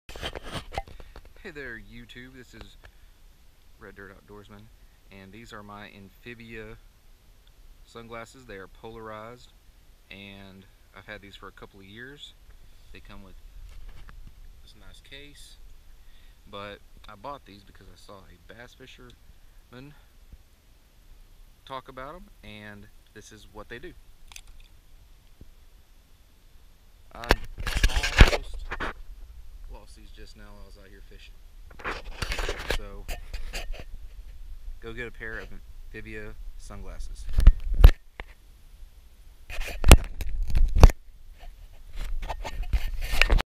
amphibia floating/polarized sunglasses
I bought these because they claim to float, and I've lost glasses in the water before.  It's been a couple of years and I've dropped them in the ocean, a couple of lakes, and I made the video because they came off while kayak fishing, and I actually had the camera with me.